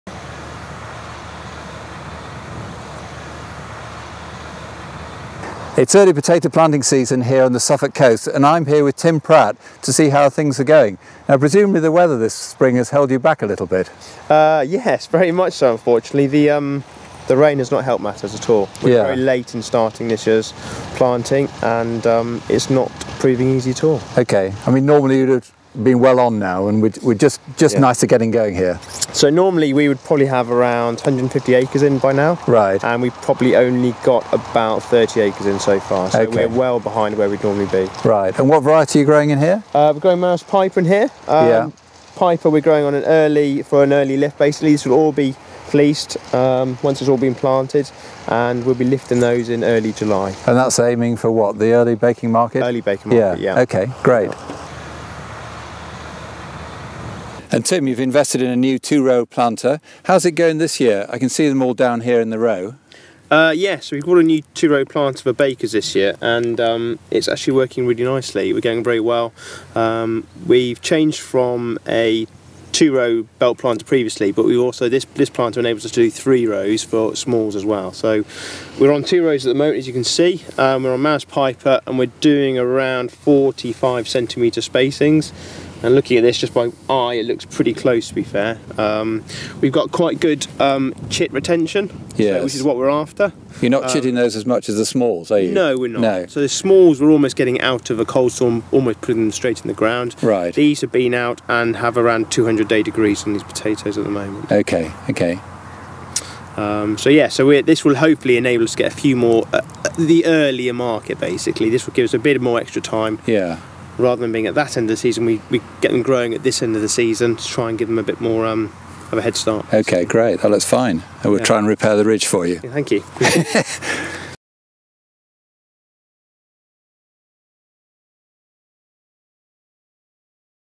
Potato planting 2018 kicks off on Tim Pratt's farm in Suffolk
Potato planting has finally kicked off on the lightest land in Suffolk - the latest start in almost two decades. Farm manager Tim Pratt is also testing out a new piece of kit for planting both two and three rows of early potatoes.